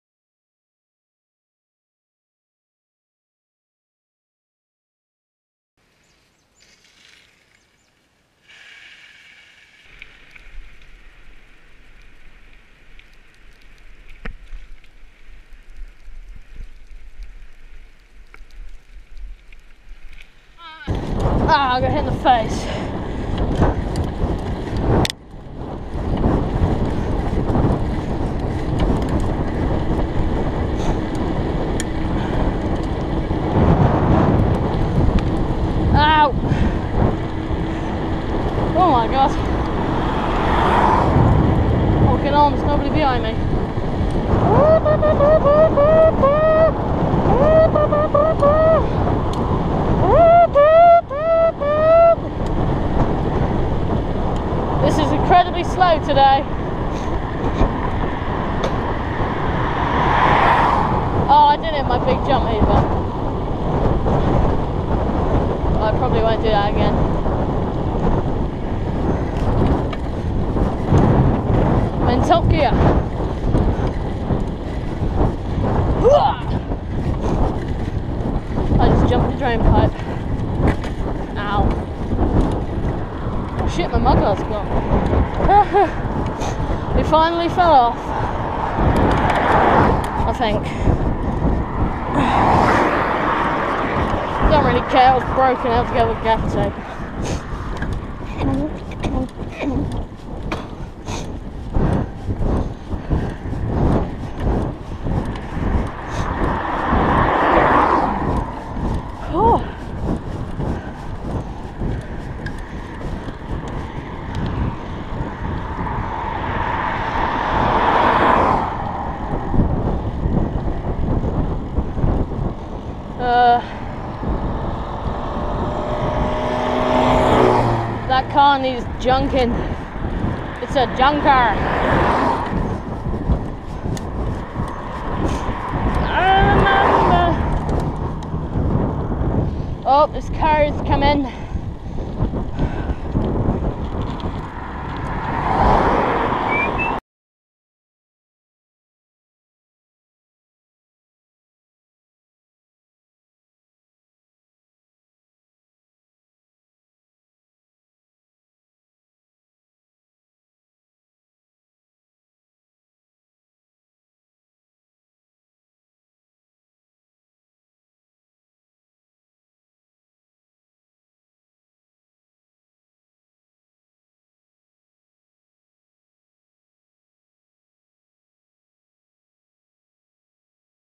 Introduction to JOHNSON RACING season 1 episode1
hello and welcome to the first ever video of mypete1963
i will be making videos and maybe commentry videos of enduro,motocross,mini moto and mtb
this episode is a short vid of mtb of just me trying to figure out the editing programme
you wont be sorry